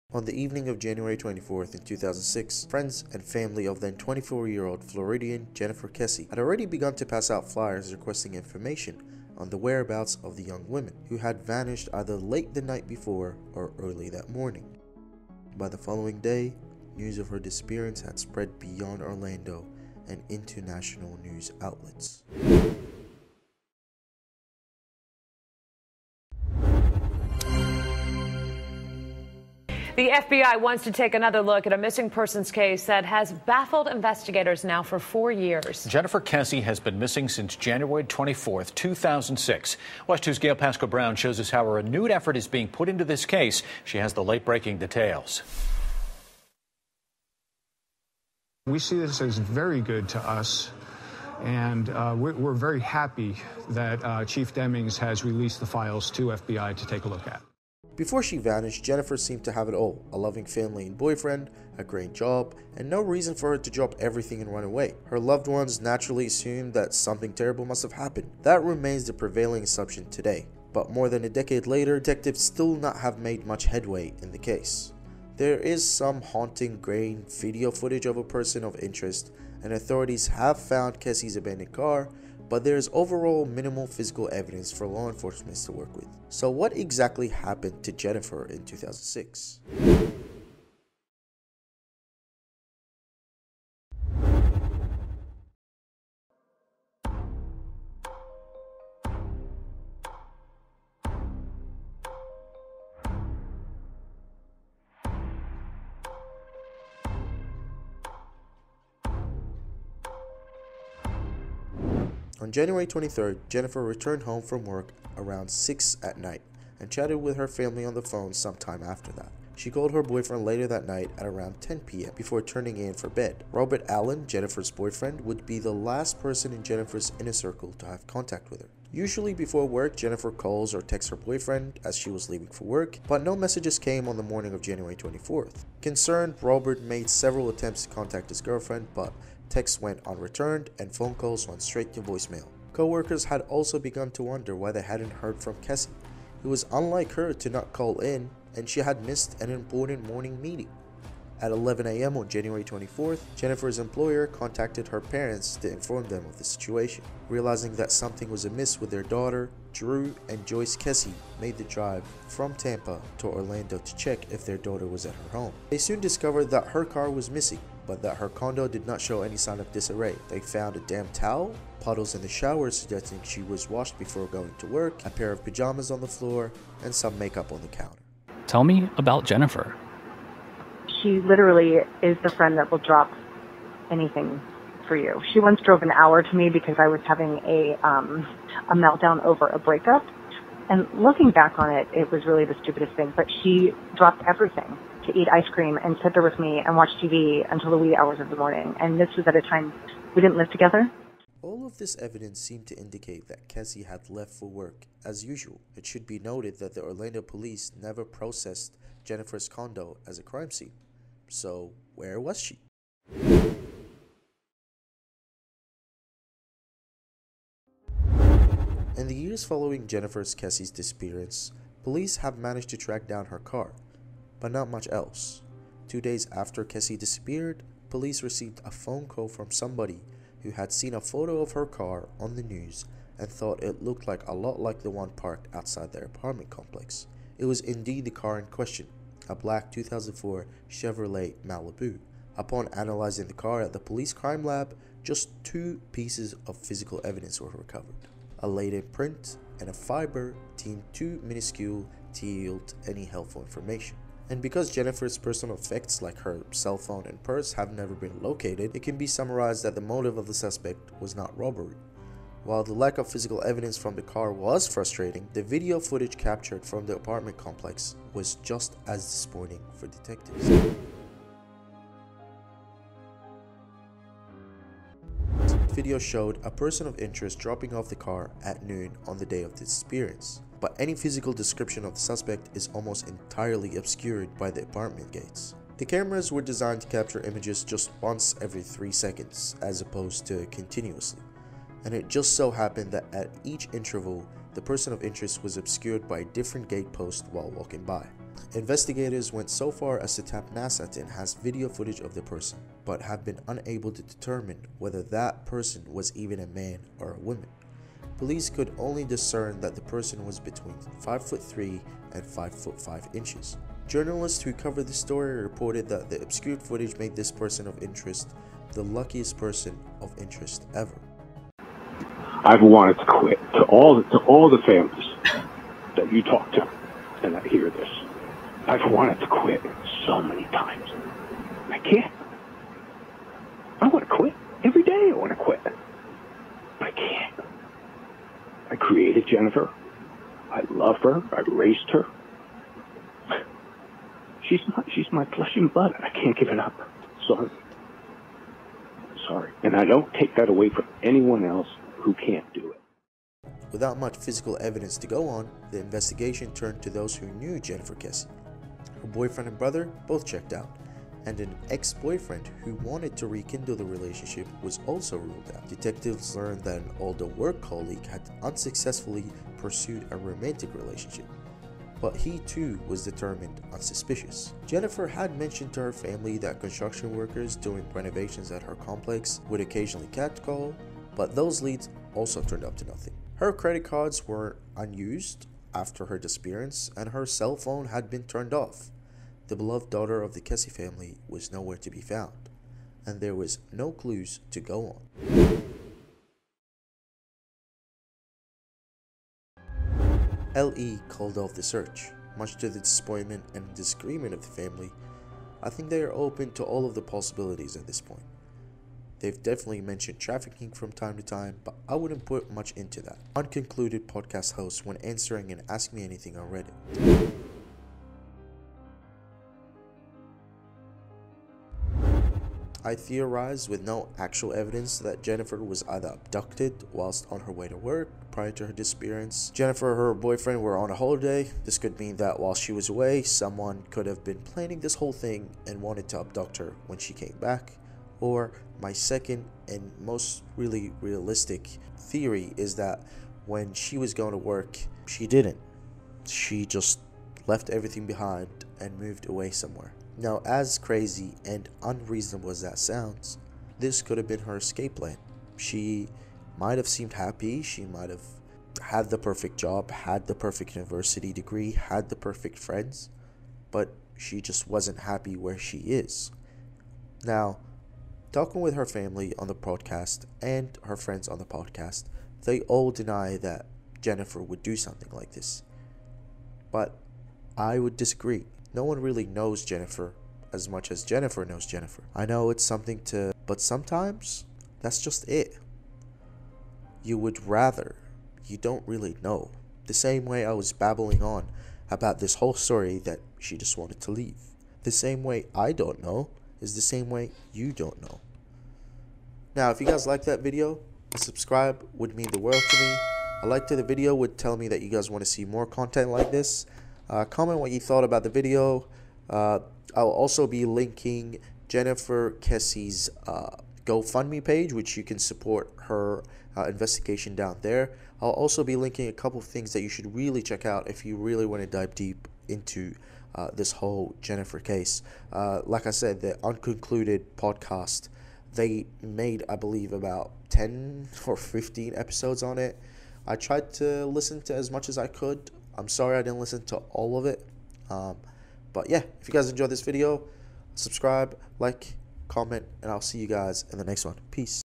This Woman Vanished without a trace | The True Story of Jennifer Kesse | 16 Years Later
Hello Welcome back to the channel today we are looking at the strange disappearance of Jennifer Kesse, Below are some helpful links to the full story if I missed any details or if you would like to read/listen to more of the story